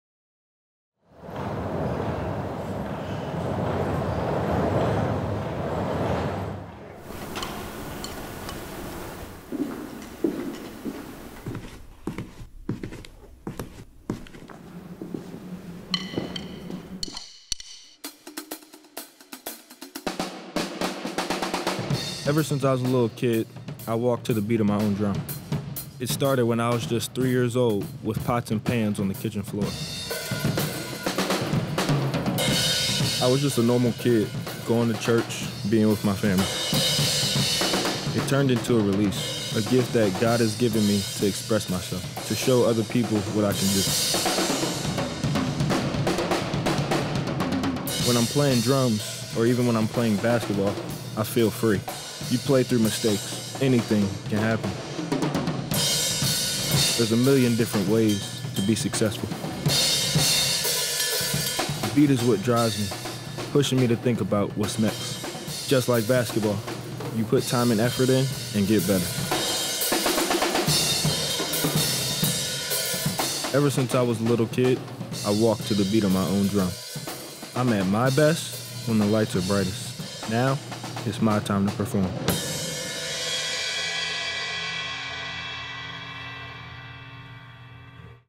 MBB: Immanuel Quickley - Time to Perform
Immanuel Quickley opened up to our crew about his passion.